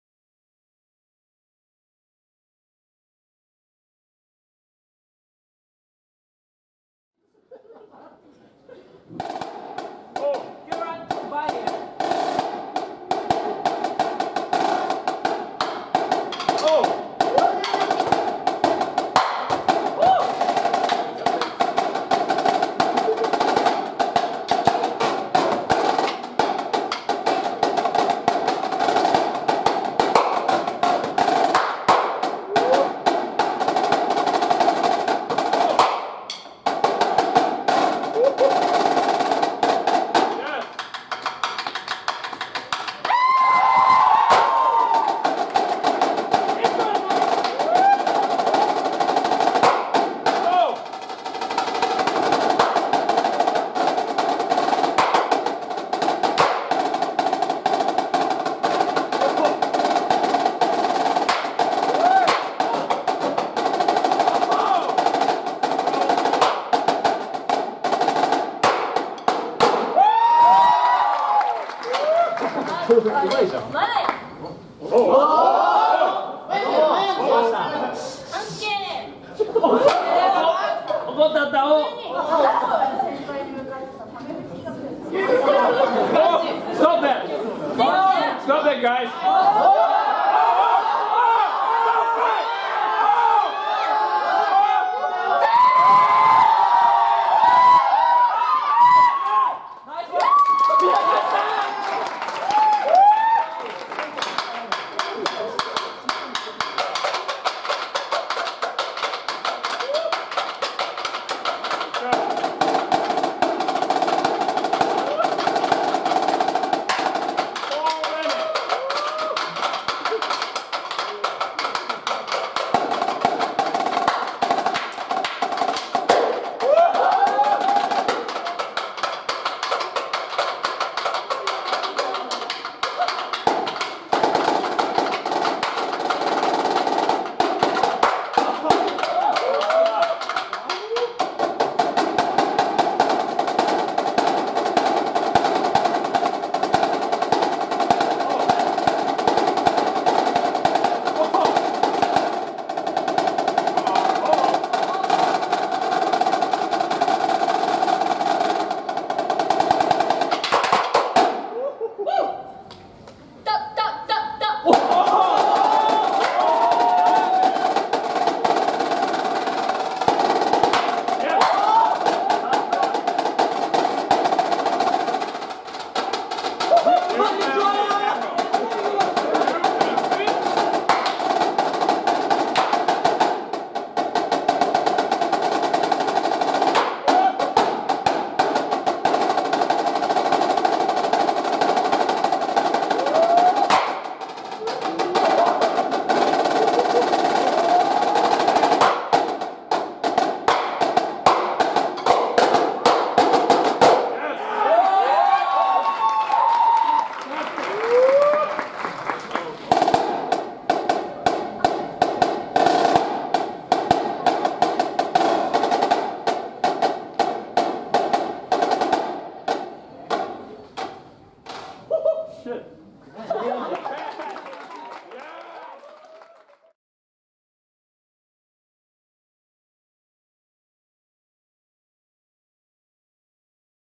2019 DCJ I&E Percussion Ensemble SD 代表者 : 鈴木有加
2019 DCJ I&E Percussion Ensemble Contest

2019年2月2日（土） 横浜文化体育館

 ■2019 DCJ I&E Percussion Ensemble
2nd   87.0
Snare Drum
THE YOKOHAMA SCOUTS
代表者 : 鈴木有加

ご協力頂きましたみなさま、ありがとうございました。